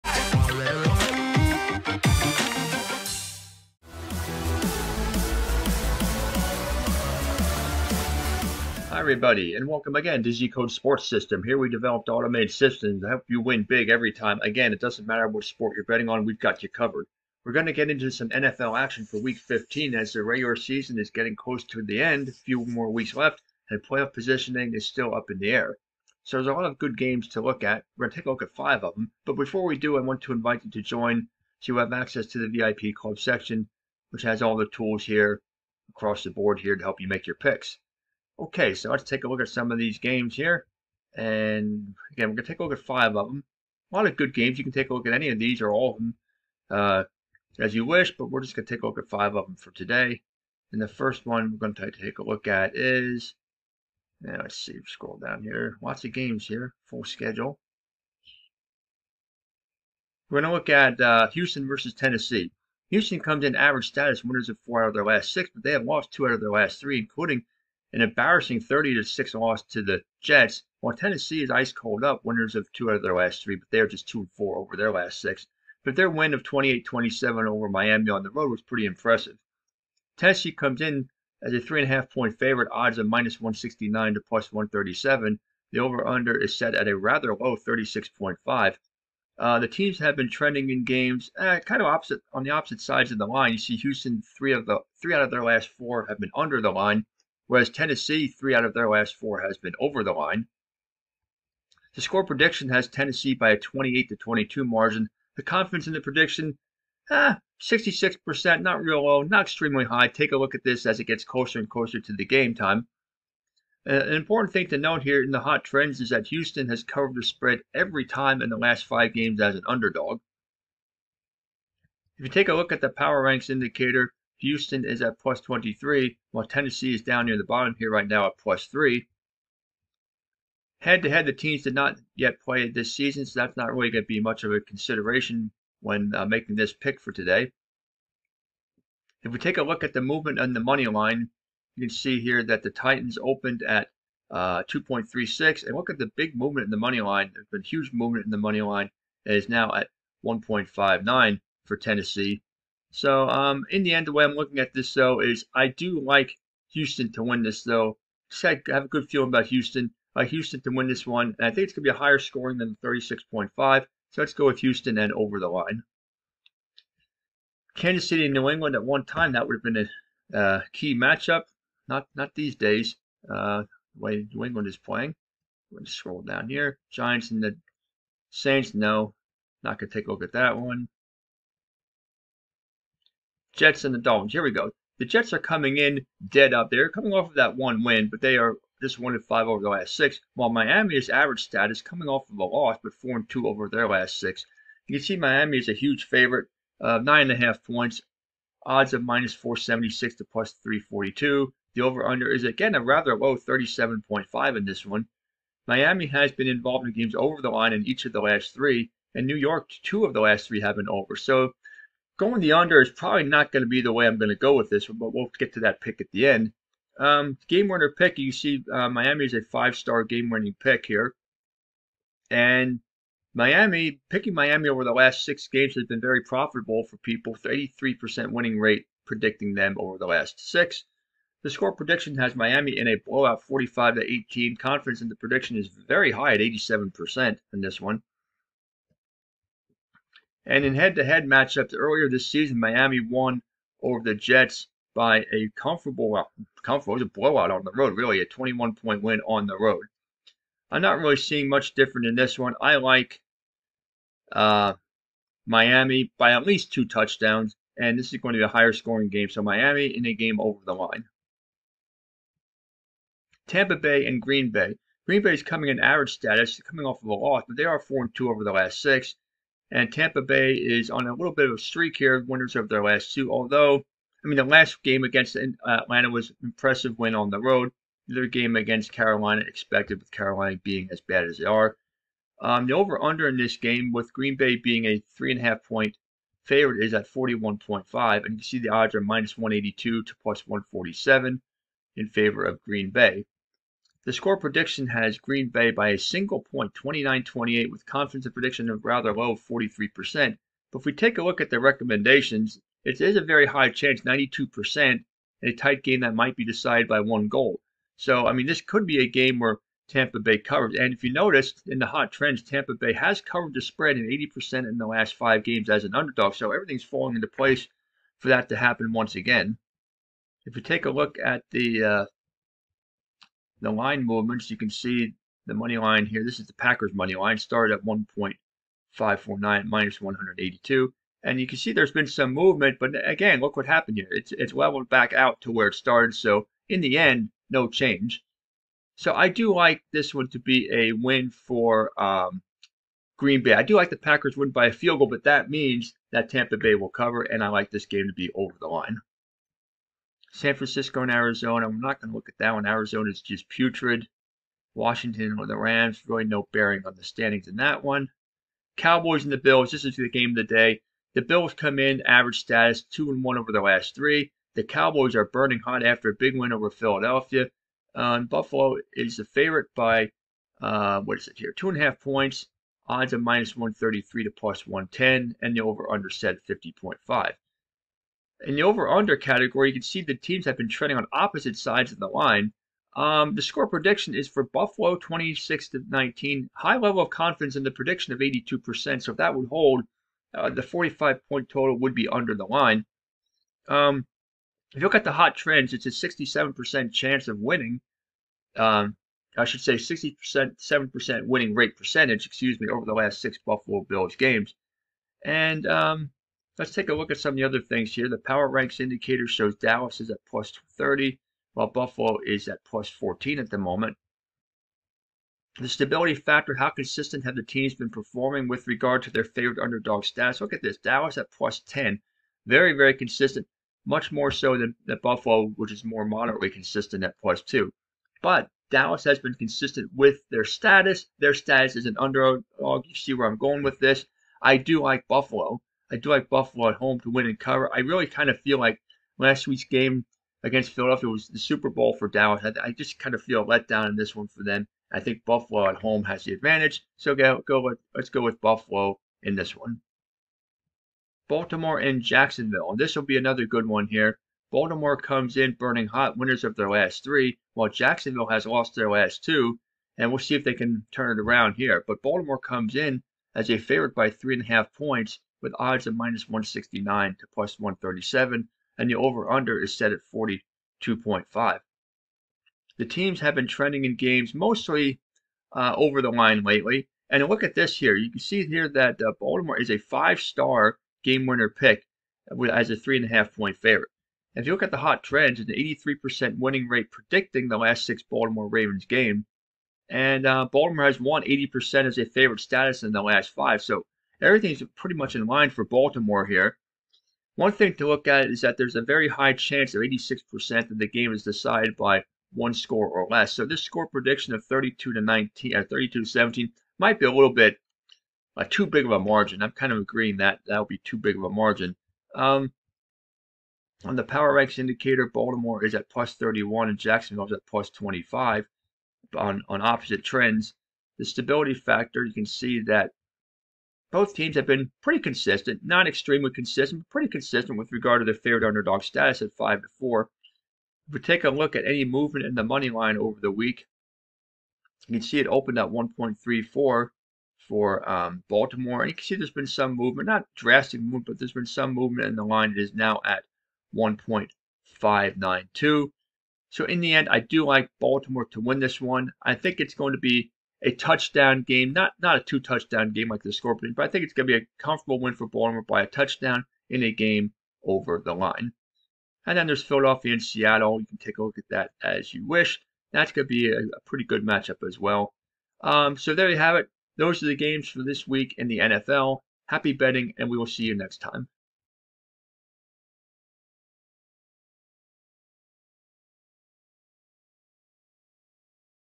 NFL Picks Week 15: Bills host Cowboys in headlined matchup
The NFL season has reached the home stretch and playoffs positions and seedings are still up in the air. There are several key matchups for week 15 with the Bills hosting the Cowboys the featured game.
🏈 Find out the NFL Picks here!